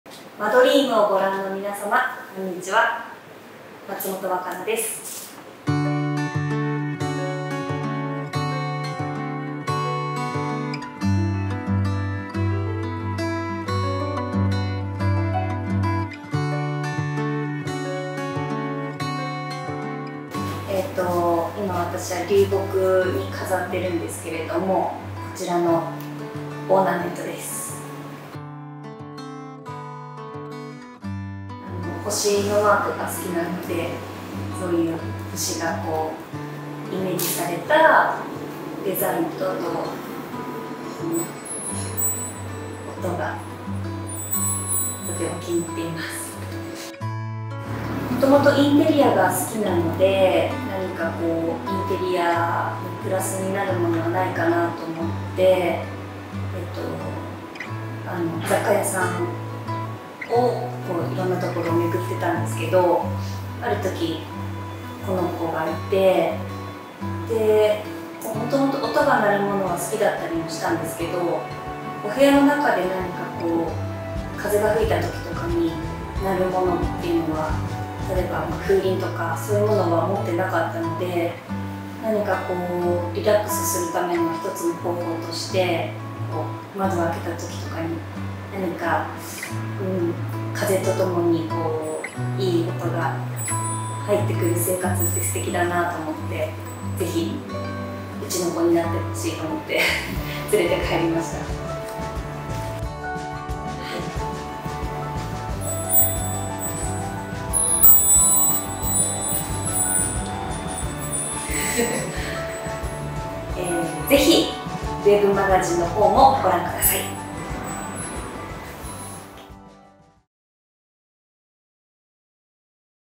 【愛用品紹介】あの人のお気に入り｜松本若菜さん｜マドリーム
▼インタビュー本文はこちらから

ライフスタイルマガジン「マドリーム」Vol.45に松本若菜さんが登場。

愛用品は、星の飾りが可愛いオーナメント。素敵な音をぜひ聞いてください。

▼松本若菜　WAKANA MATSUMOTO
1984年、鳥取県生まれ。2007年女優デビュー。2009年に映画「腐女子彼女。」で初主演をつとめる。2017年に映画「愚行録」でヨコハマ映画祭助演女優賞を受賞。2022年のドラマ「やんごとなき一族」（フジテレビ系）での怪演が“松本劇場”と一躍話題に。初主演連続ドラマ「復讐の未亡人」（テレビ東京系）が放映中、8月25日（木）に最終回を迎える。